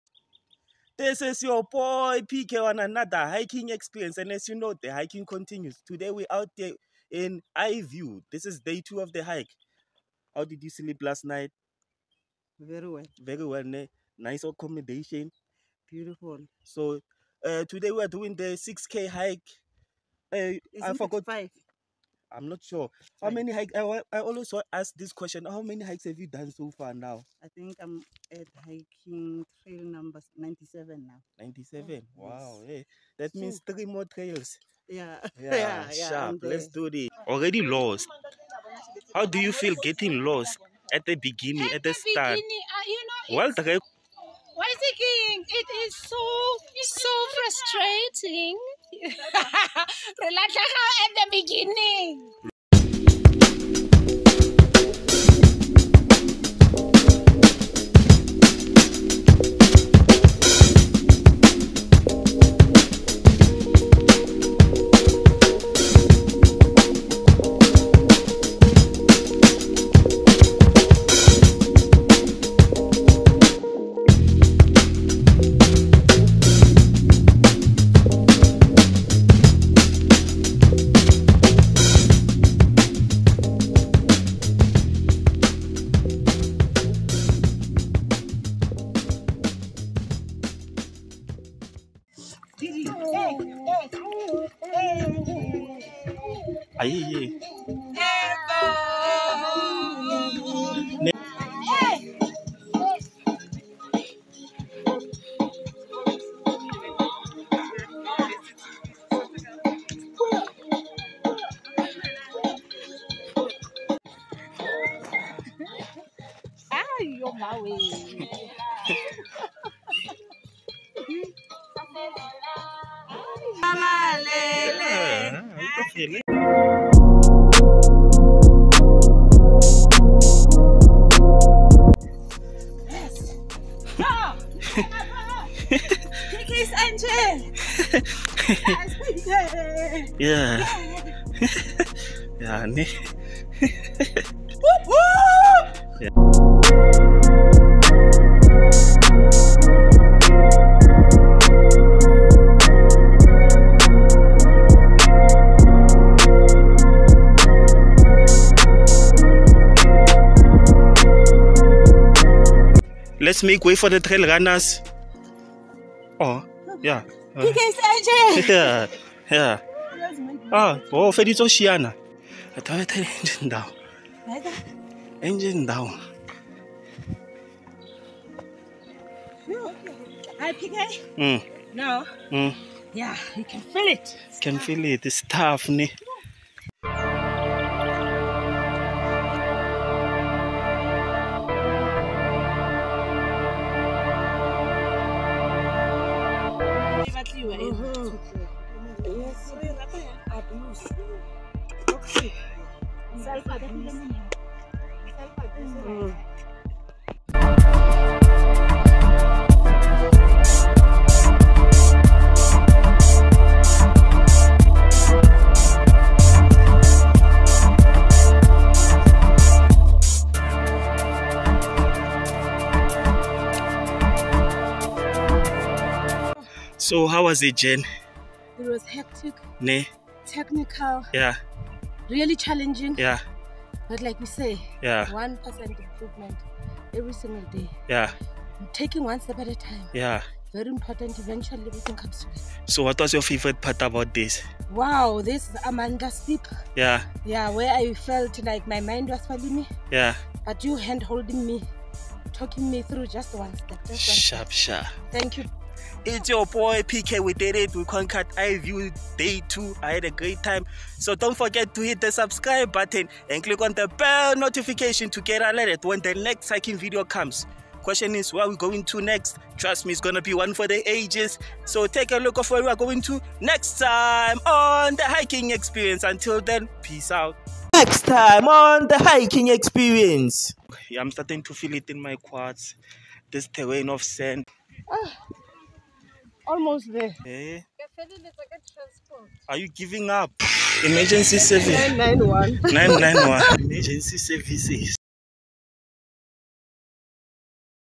The Hiking Experience E70: Eye View continues
In our 2nd day at Eye View Game Farm..we tackle the short 5km...So lets see what the trail has in store for us